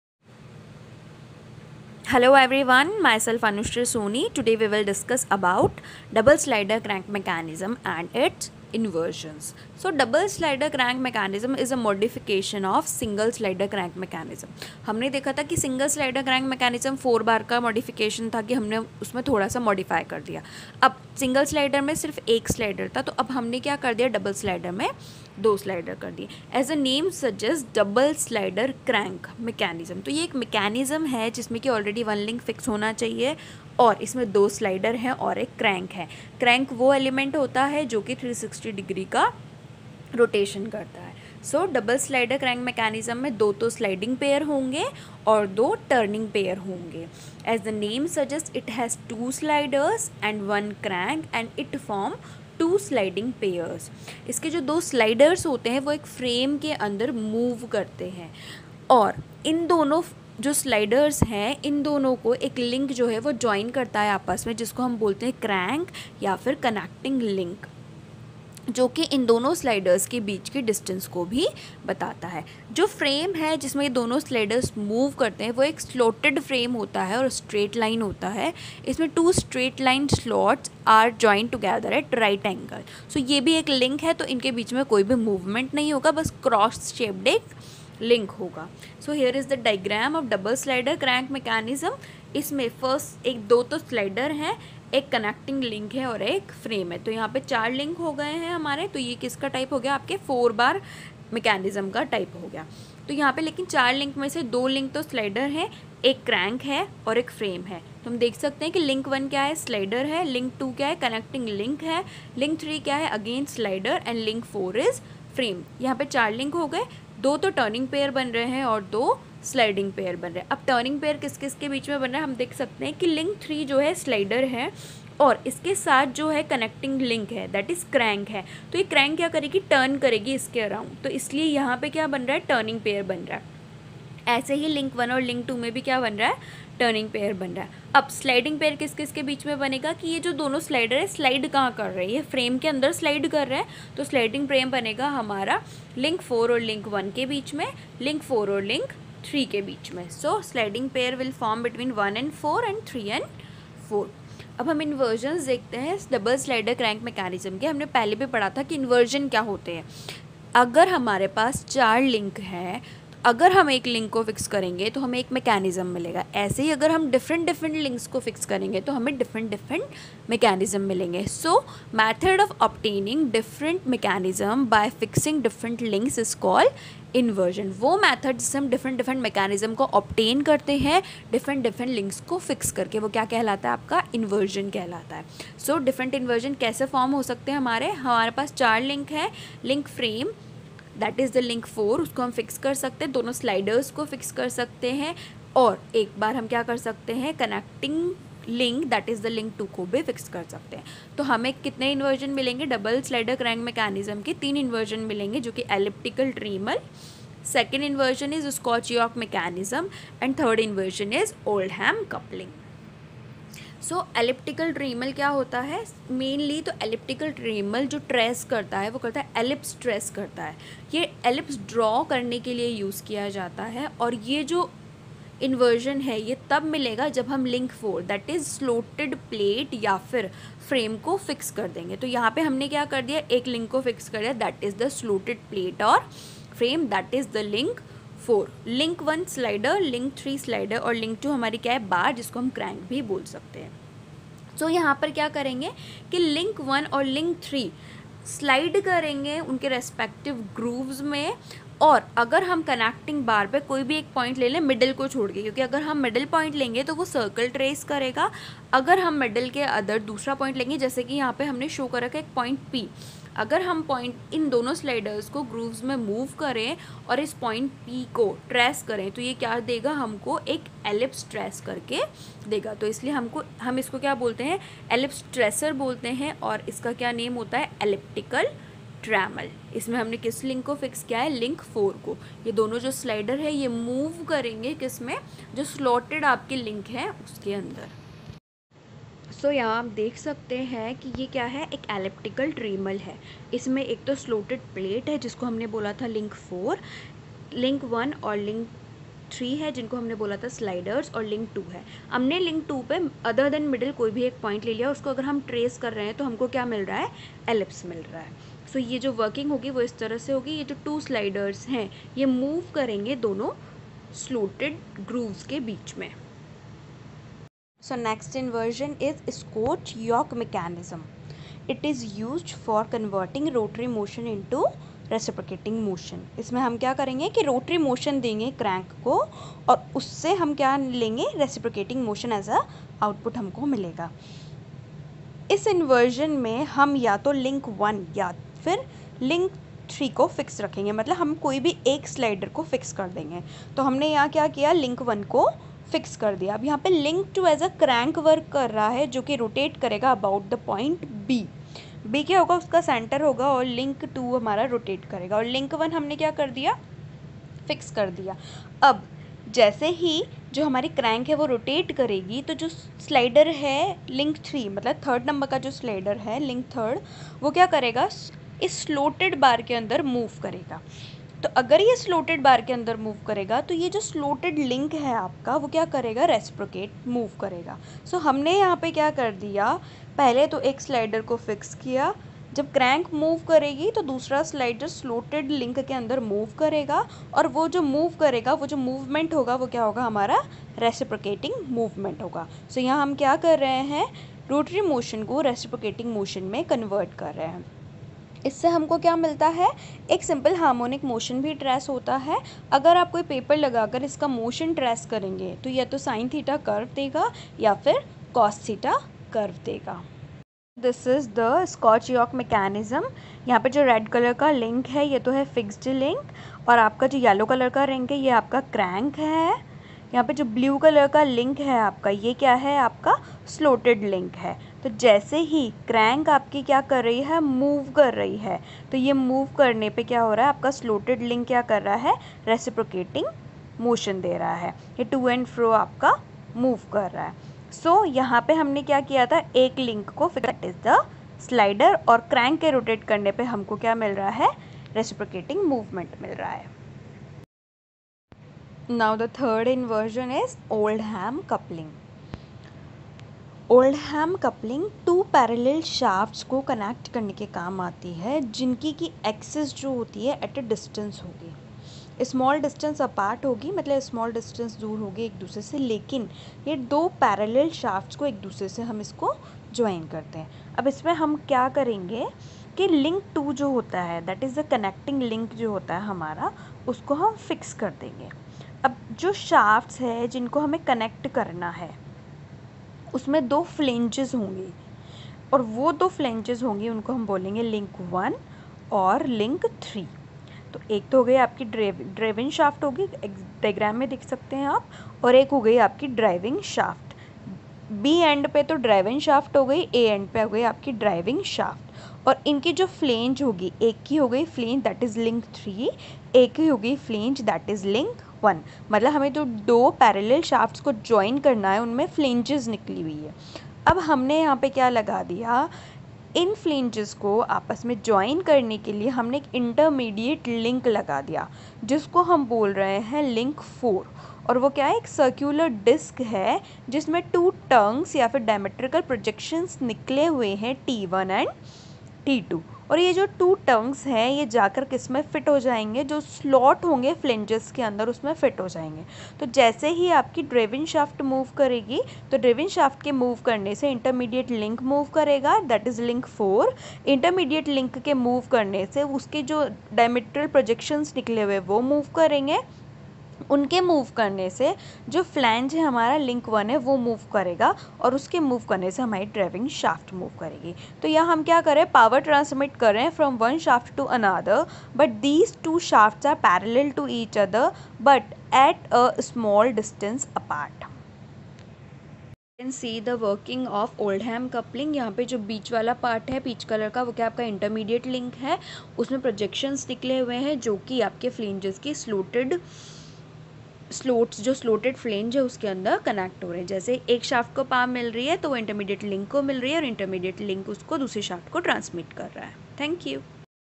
Double Slider Crank Mechanism and It's Inversions - TOM Lecture-6 by Anushree Soni
This Video Covering Points - (i) What is Double Slider Crank Mechanism
(ii) Inversion of Double Slider Crank Mechanism and its working
(iii) Elliptical Trammel and its working
(iii) Scotchyoke Mechanism and its working
(iv) Oldham Coupling and its working
All these points explained by some good practical experiments here to get you learn in simplest way